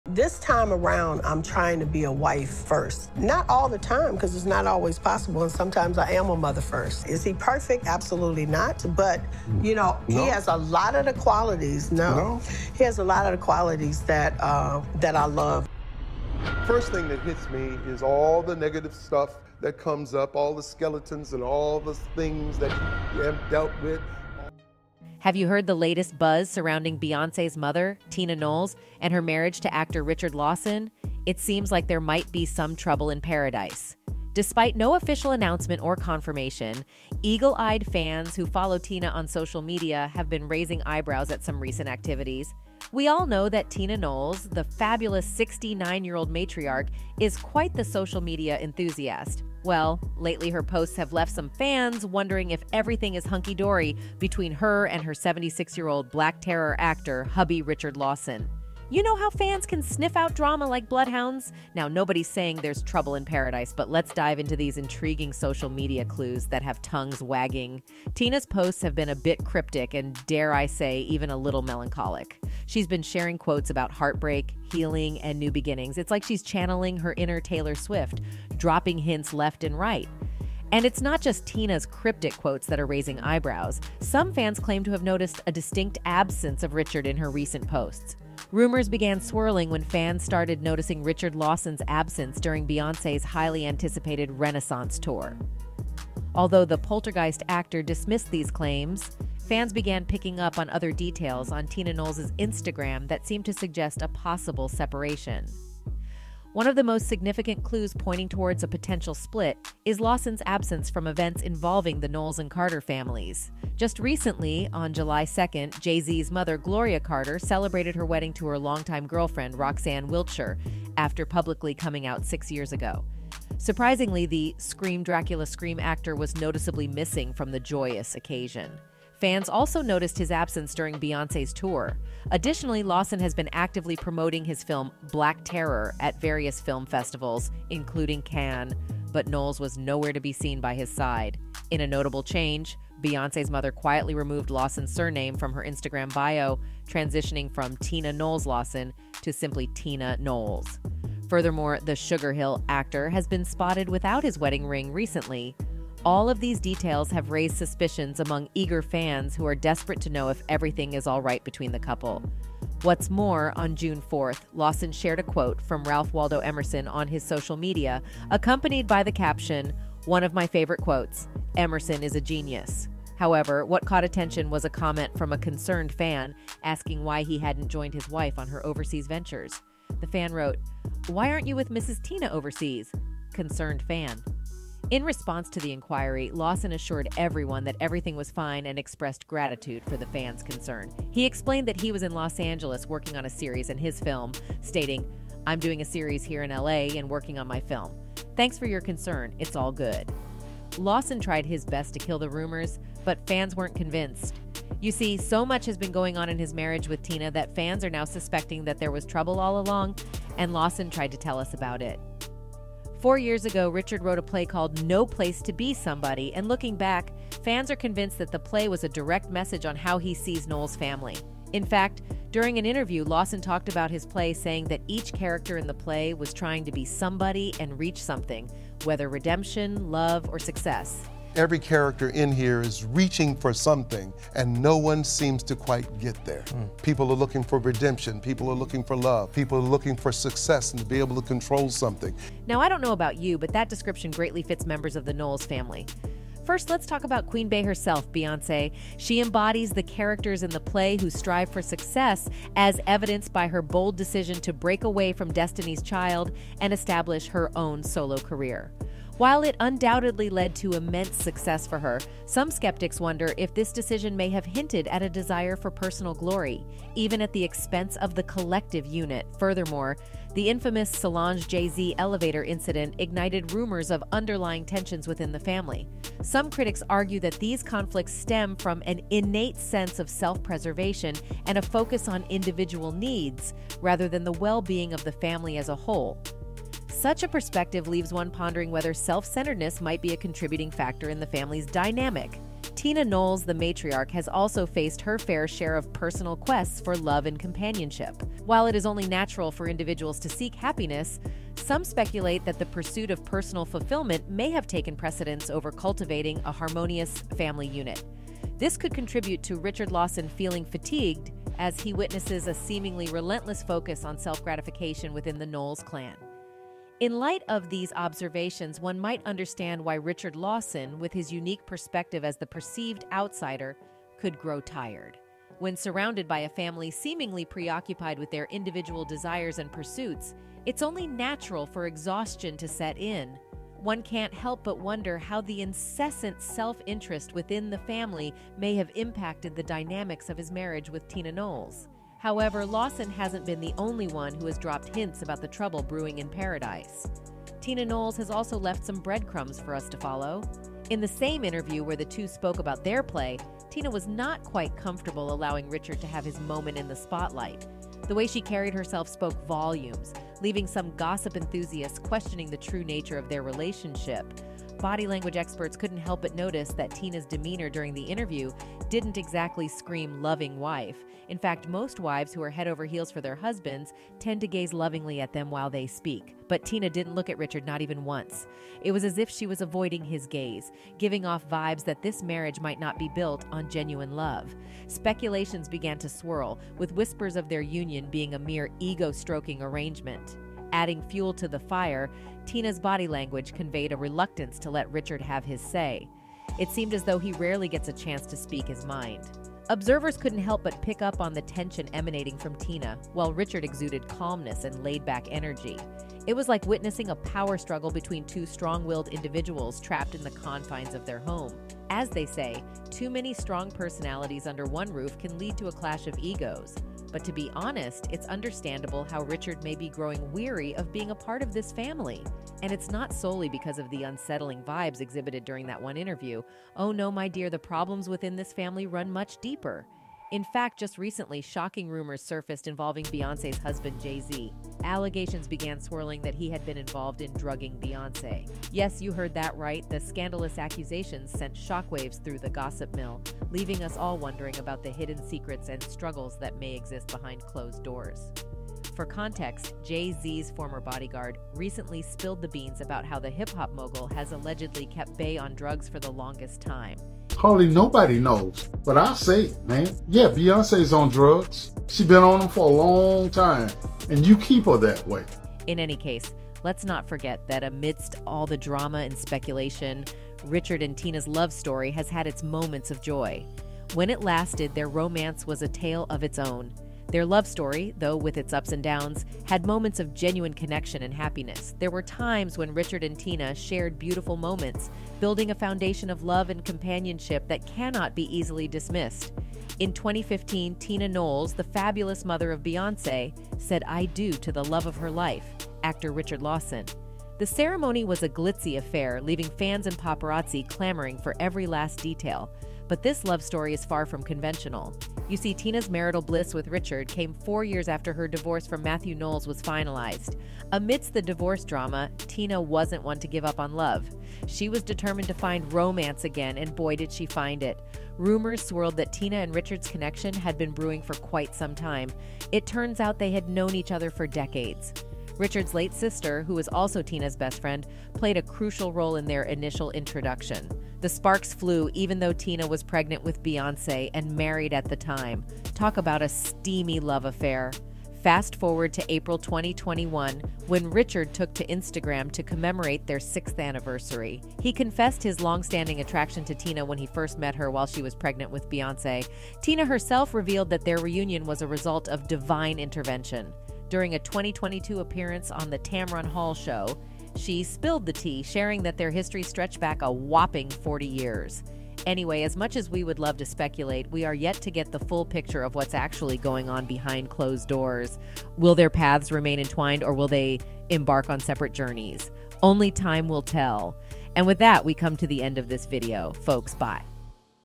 7 MINUTES AGO: Richard Lawson Reveals He’s Had Enough Knowles FOR LIFE
7 MINUTES AGO: Richard Lawson Reveals He’s Had Enough Knowles FOR LIFE.
Lawson tried his best to kill the rumors but fans weren’t convinced. You see, so much has been going on in his marriage with Tina that fans are now suspecting that there was trouble all along and Lawson tried to tell us about it.4 years ago, Richard wrote a play called No Place to be Somebody, and looking back, fans are convinced that the play was a direct message on how he sees Knowles family. In fact, during an interview, Lawson talked about his play saying that each character in the play was trying to be somebody and reach something; whether redemption, love or success.Now, I don't know about you, but that description greatly fits members of the Knowles family.First, let's talk about Queen Bey herself, Beyoncé. She embodies the characters in the play who strive for success, as evidenced by her bold decision to break away from Destiny's Child and establish her own solo career.

On Spill Today you can expect the latest updates on all trending celebrity news and gossip!